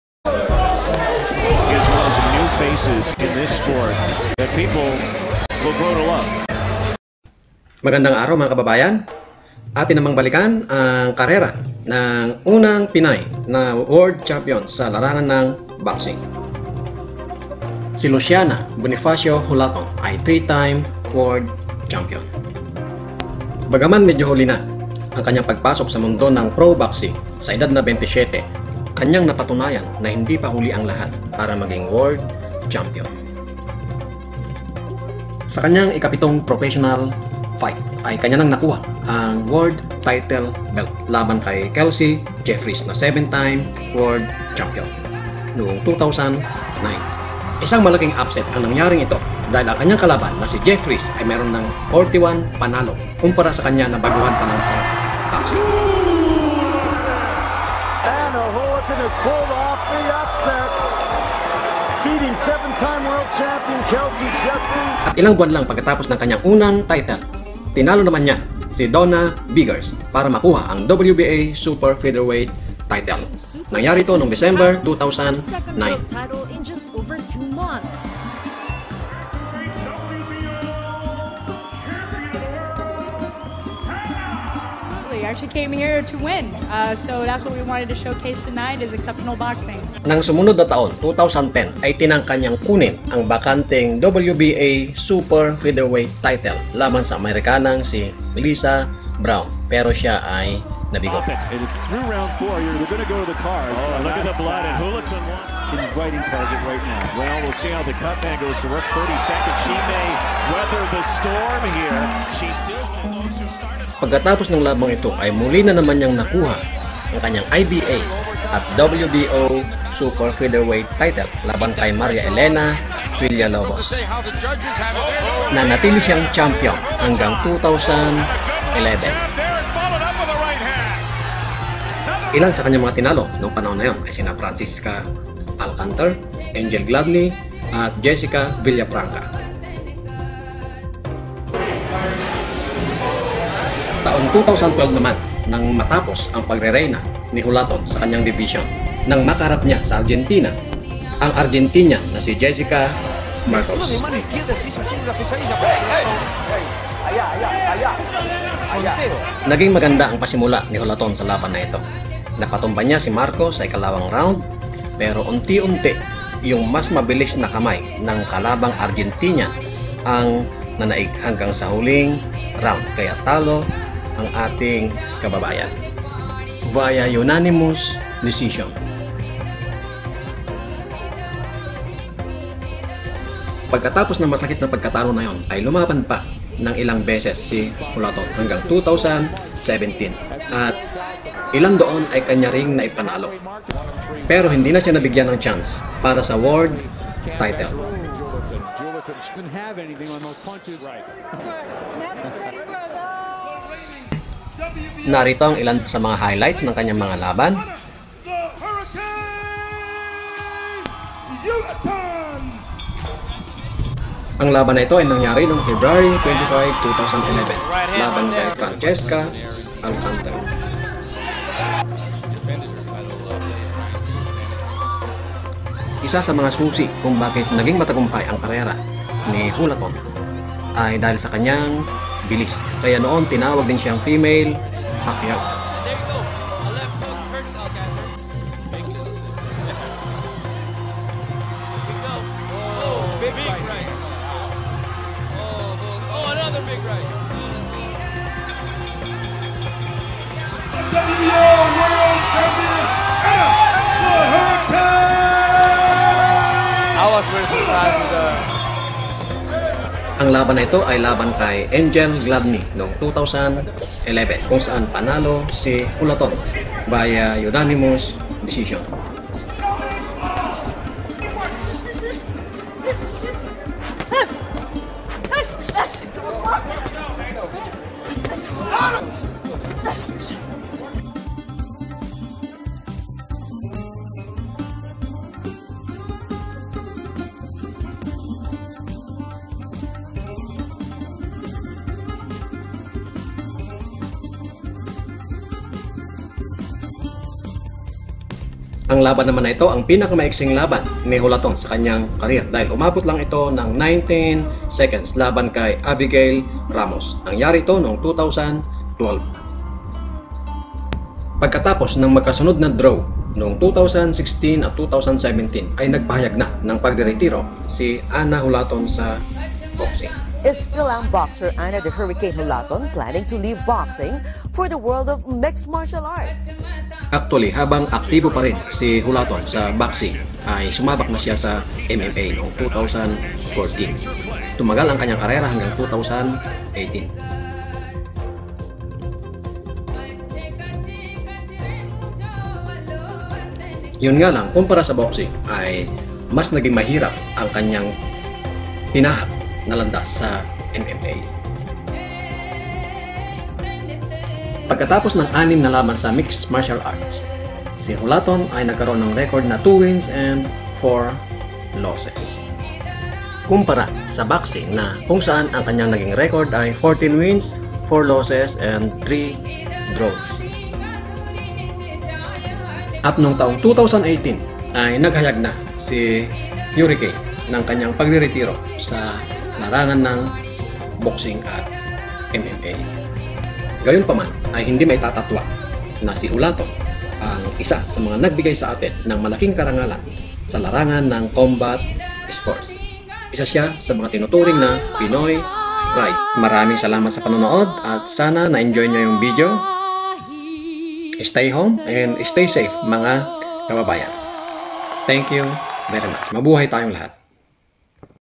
Ana The Hurricane Julaton Highlights
Female Manny Pacquiao ang tawag sa kanya noong kanyang panahon dahil sa taglay nyang bilis. At dahil din sa kanyang bilis ay naging three time world boxing champion mula 2009 hanggang 2011 sa Super Bantamweight Division ng IBA,WBA at WBO.  Sa kanyang 10 taong karerea sa boksing ay nakapagtala sya ng 14 panalo(2 KO), 4 na talo at 3 draw.